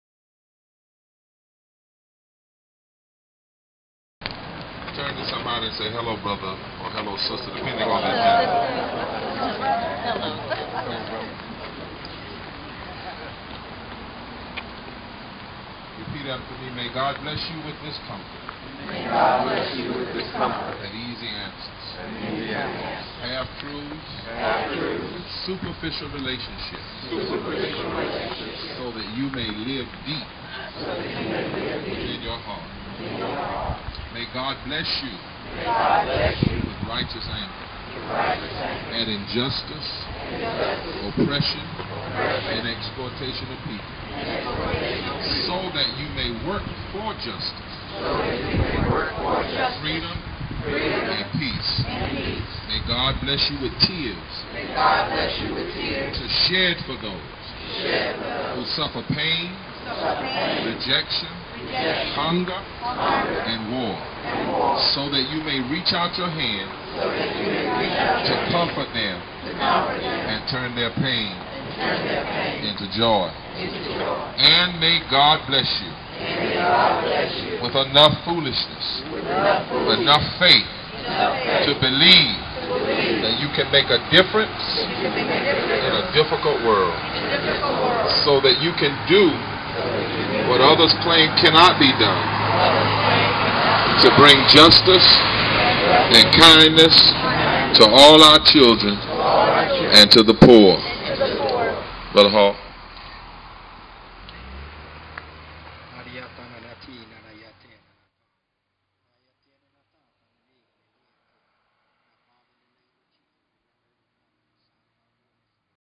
Meeting in Mount Airy
Prayer at the end of a meeting near a closed factory in Mount Airy. The words are from the Franciscan tradition.
From a series of videos taken during the Truth & Hope Tour of Poverty in North Carolina, January-May 2012